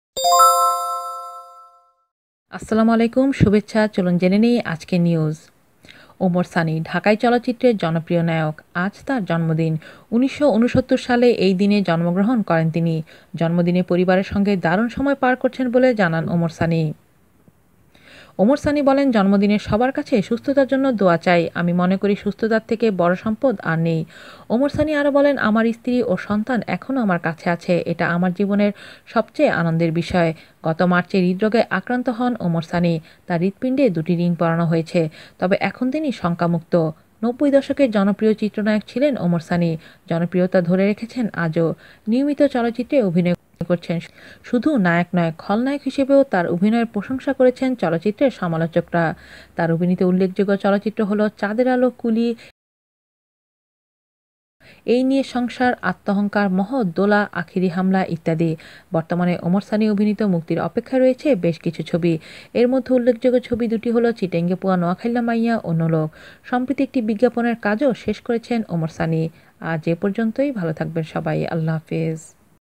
হঠাত আবার কি হলো যে সুস্থতার জন্য দোয়া চাইলেন সবার কাছে ওমর সানী !! Omar Sani!!
ওমর সানী। ঢাকাই চলচ্চিত্রের জনপ্রিয় নায়ক। আজ তাঁর জন্মদিন। ১৯৬৯ সালে এই দিনে জন্মগ্রহণ করেন তিনি।

জন্মদিনে পরিবারের সঙ্গে দারুণ সময় পার করছেন বলে জানান ওমর সানী।  এনটিভি অনলাইনকে ওমর সানী বলেন, ‘জন্মদিনে সবার কাছে সুস্থতার জন্য দোয়া চাই। আমি মনে করি, সুস্থতার থেকে বড় সম্পদ আর নেই। ’

ওমর সানী আরও বলেন, ‘আমার স্ত্রী ও সন্তান এখনো আমার কাছে আছে। এটা আমার জীবনে সবচেয়ে আনন্দের বিষয়।’

গত মার্চে হৃদরোগে আক্রান্ত হন ওমর সানী। তাঁর  হৃৎপিণ্ডে  দুইটি রিং পরানো হয়েছে। তবে এখন  তিনি শংকামুক্ত।